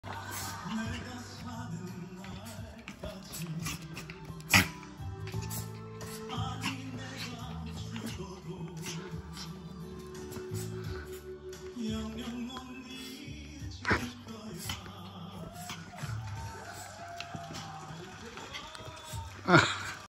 광견병에 걸린 척하는 강아지
말티폼의 일반적인 저녁 일상